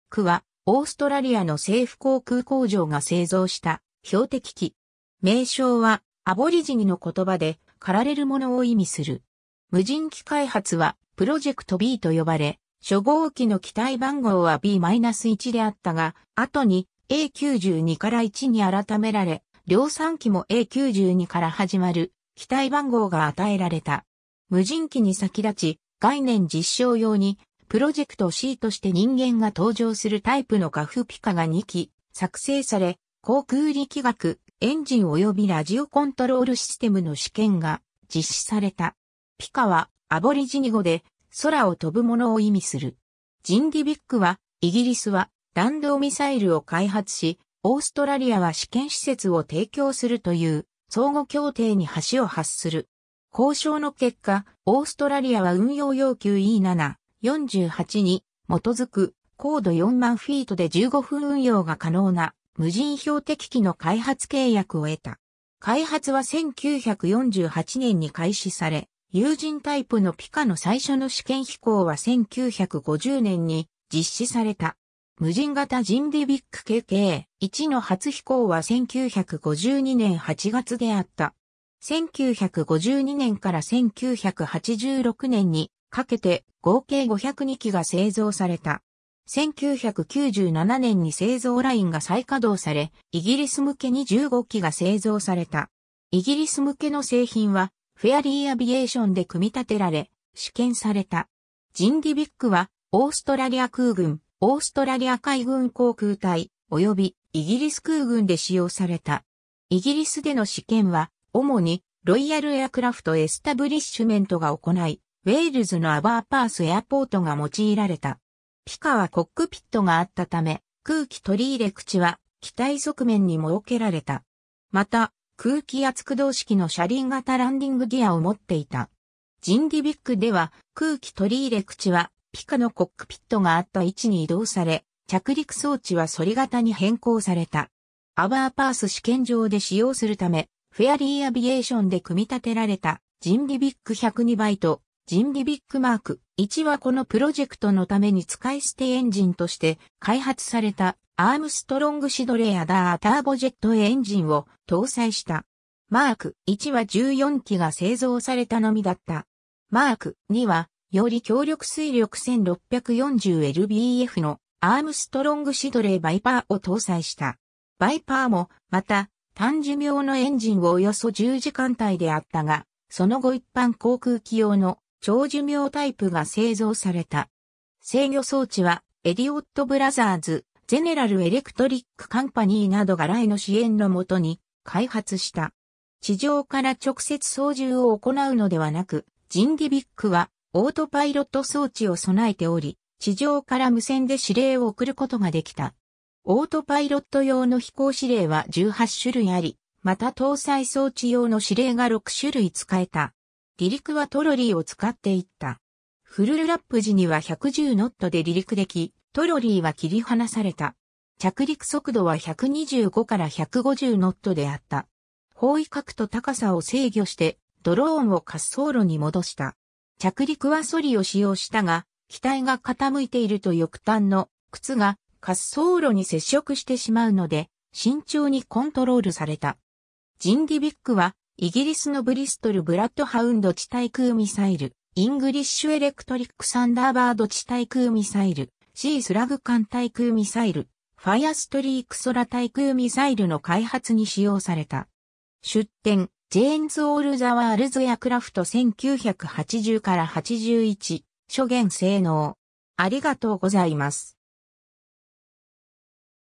GAF ジンディビック
GAF ジンディビック GAF ジンディビック（GAF Jindivik）はオーストラリアの政府航空工場（Government Aircraft Factory）が製造した標的機。
名称はアボリジニの言葉で「狩られるもの」を意味する。
無人機開発はProject Bと呼ばれ、初号機の機体番号はB-1であったが、後にA92-1に改められ、量産機もA92から始まる機体番号が与えられた。
無人機に先立ち、概念実証用に、Project Cとして人間が搭乗するタイプのGAF Pika が2機作製され（Aircraft C-1およびC-2、後に93-1及び93-2と改称）、航空力学、エンジンおよびラジオコントロールシステムの試験が実施された。
Pikaはアボリジニ語で「空をとぶもの」を意味する。
 ジンディビックは、イギリスは弾道ミサイルを開発し、オーストラリアは試験施設（後のウーメラ試験場）を提供するという相互協定に端を発する。
交渉の結果、オーストラリアは運用要求 E.7/48 に基づく、高度40,000 ftで15分運用が可能な無人標的機の開発契約を得た。
開発は1948年に開始され、有人タイプのPikaの最初の試験飛行は1950年に実施された。
無人型ジンディビック Kk.1 の初飛行は1952年8月であった。
1952年から1986年にかけて、合計502機が製造された。
1997年に製造ラインが再稼動され、イギリス向けに15機が製造された。
イギリス向けの製品は、フェアリー・アビエーションで組み立てられ、試験された。
ジンディビックはオーストラリア空軍、オーストラリア海軍航空隊、及びイギリス空軍で使用された。
イギリスでの試験は、主にロイヤル・エアクラフト・エスタブリッシュメント（RAE）が行ない、ウェールズのAberporth Airportが用いられた。
 Pikaはコックピットがあったため、空気取り入れ口は機体側面に設けられた。
また空気圧駆動式の車輪型ランディング・ギアを持っていた。
ジンディビックでは、空気取り入れ口はPIKAのコックピットがあった位置に移動され、着陸装置はソリ型に変更された。
 Aberporth試験場で使用するため、フェアリー・アビエーションで組み立てられたジンディビック 102B ジンディビック Mk.1 はこのプロジェクトのために使い捨てエンジンとして開発されたアームストロング・シドレー アダー ターボジェットエンジンを搭載した。
Mk.1 は14機が製造されたのみだった。
Mk.2 はより強力推力1,640 lbfのアームストロング・シドレー ヴァイパー を搭載した。
ヴァイパーもまた、短寿命のエンジン - およそ10時間 - であったが、その後一般航空機用の「長寿命タイプ」が製造された。
 制御装置はエリオット・ブラザーズ、ゼネラル・エレクトリック・カンパニーなどがRAEの支援のもとに開発した。
地上から直接操縦を行うのではなく、ジンディビックはオートパイロット装置を備えており、地上から無線で指令を送ることができた。
オートパイロット用の飛行指令は18種類あり、また搭載装置用の指令が6種類使えた。
 離陸はトロリーを使って行った。
フルルラップ時には110ノットで離陸でき、トロリーは切り離された。
着陸速度は125-150ノットであった。
方位角と高さを制御して、ドローンを滑走路に戻した。
着陸はソリを使用したが、機体が傾いていると翼端の「靴」が滑走路に接触してしまうので、慎重にコントロールされた。
 ジンディビックは、イギリスのBristol Bloodhound 地対空ミサイル、English Electric Thunderbird 地対空ミサイル、シー・スラグ艦対空ミサイル、ファイアストリーク 空対空ミサイルの開発に使用された。
 出典: Jane's All The World's Aircraft 1980-81 諸元 性能